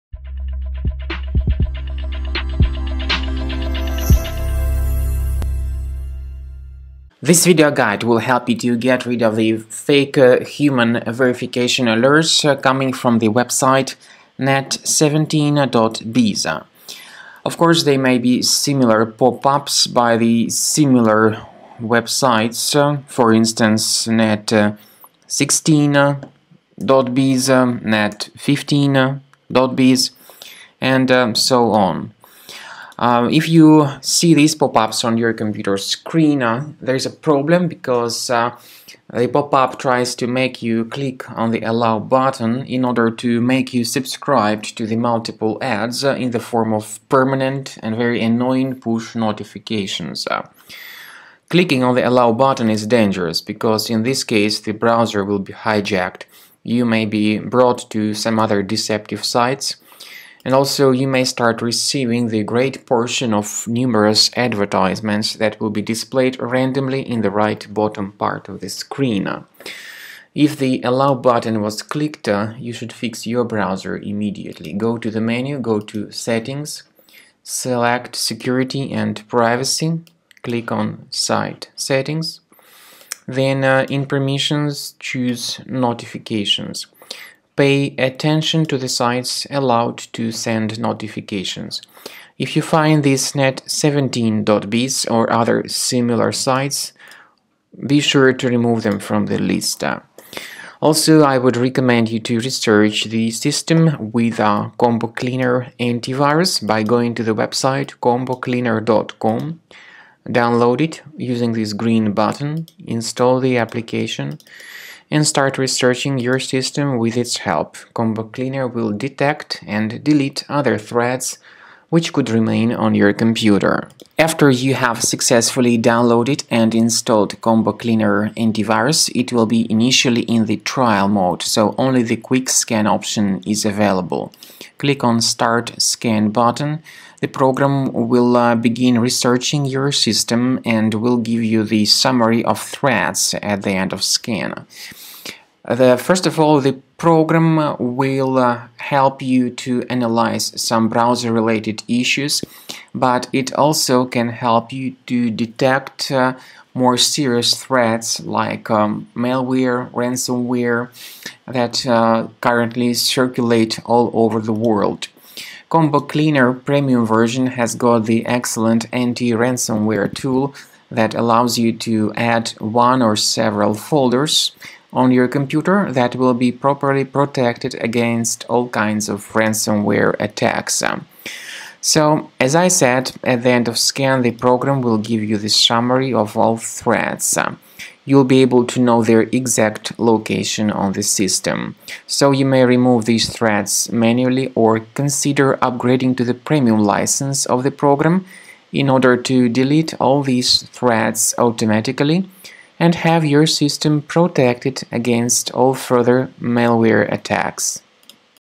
Net17.biz advertisements - how to remove?
Net17.biz is a dangerous site that displays multiple advertisements in the form of annoying web push notifications. You should not click on the "Allow" button as these pop-ups suggest. Otherwise, these ads will keep attacking your screen regularly. Get rid of them all with the help of Combo Cleaner Antivirus or follow the manual guidelines set forth in the video.